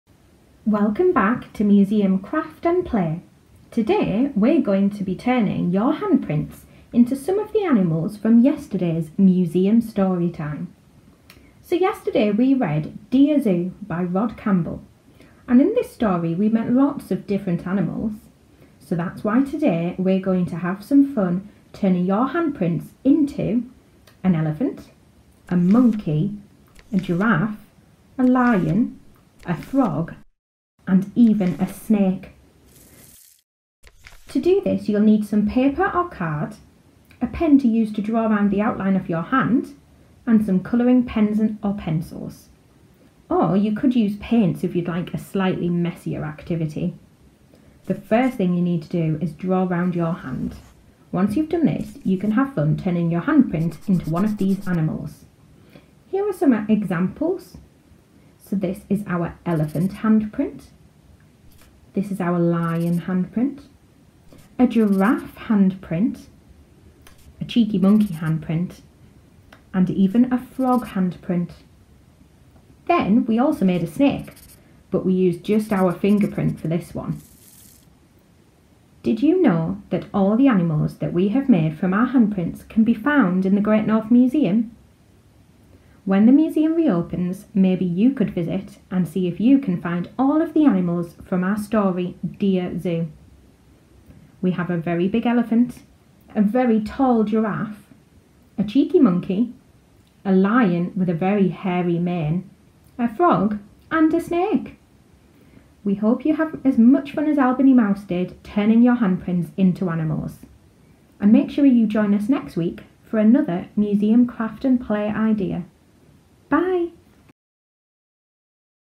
Craft with Albany Mouse: Dear Zoo Handprint Animals
Craft with Albany the Great North Mouse. Albany is the mascot of the Great North Museum under 5s programme.